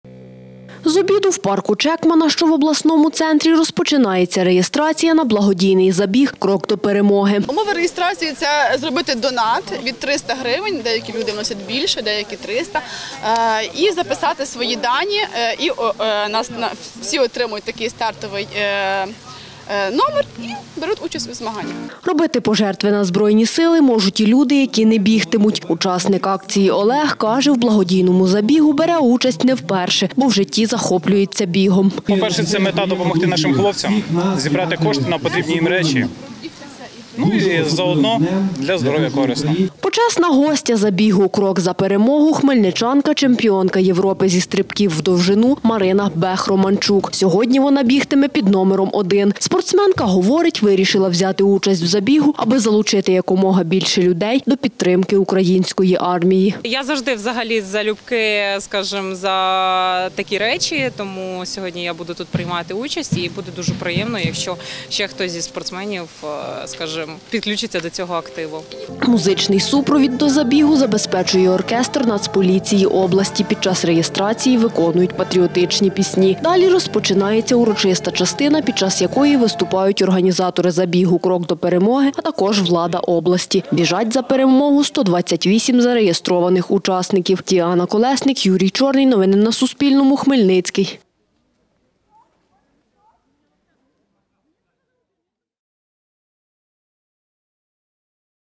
У Хмельницькому відбувся благодійний забіг «Крок за перемогу»
128 людей сьогодні взяли участь в благодійному забігу «Крок за перемогу». Як розповіла Суспільному співорганізаторка забігу Сніжана Кріль, хмельничани, які бігли в парку Чекмана, робили благодійні внески від 300 гривень на потреби військових формувань Хмельницької області.